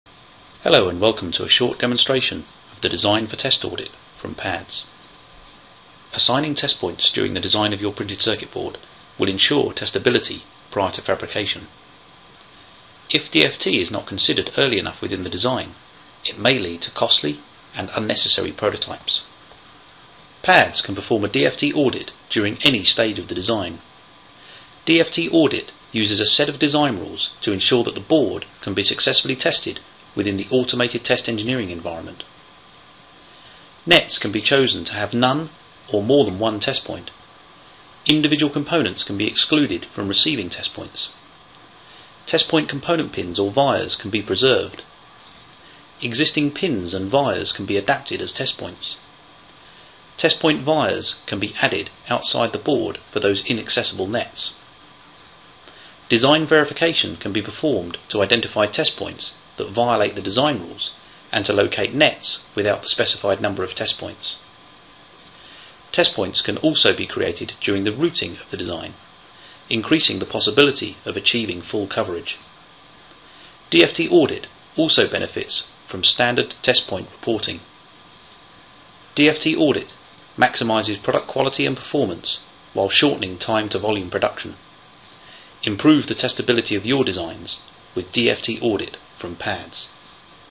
PADS DFT Audit - Test Points
PADS DFT Audit is an embedded design for test auditing tool that allows you to perform DFT analysis and verification before releasing the PCB for fabrication.